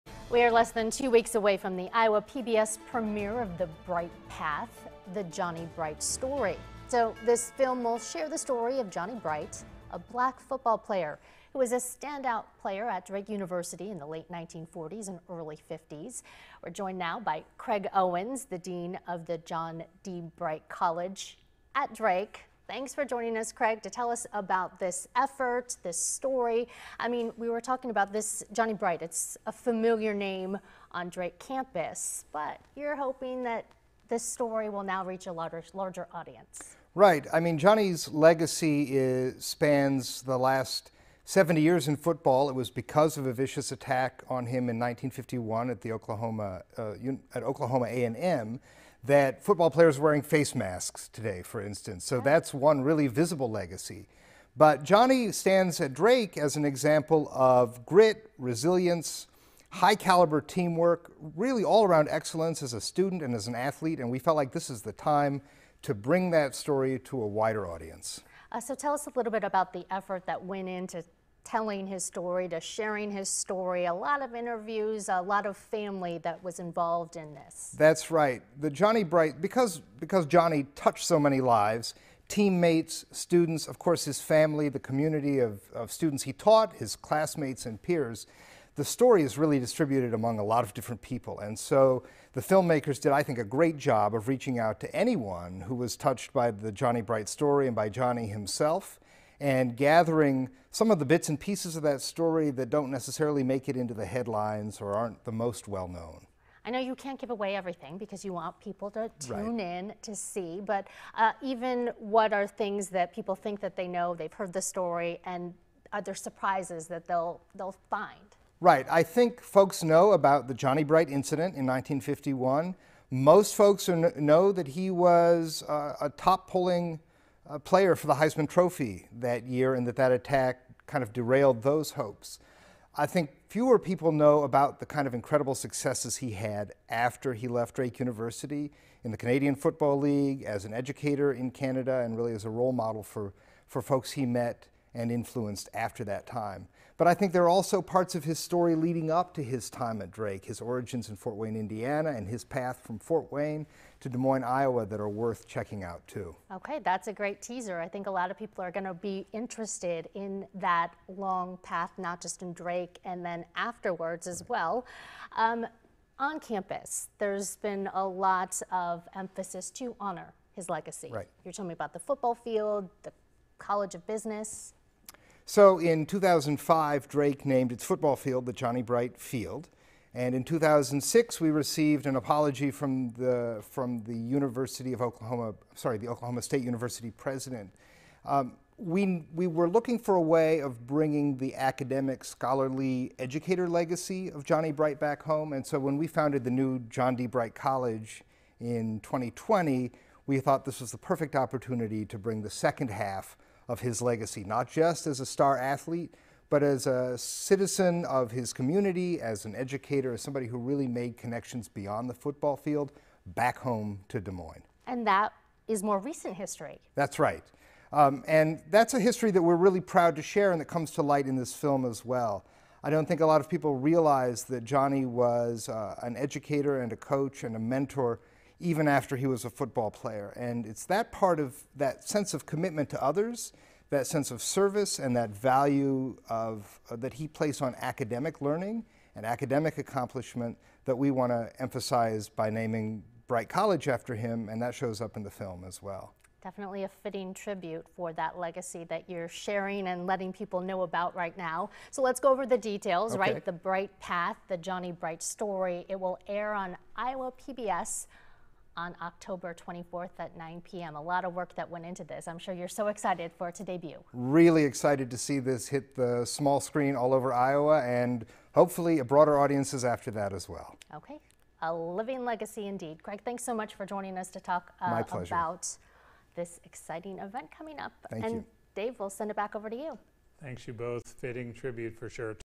Former Drake University star quarterback to be honored in new documentary
A former star quarterback, known for his athletic and academic talent, at Drake University will have his story told in a new documentary on Iowa PBS.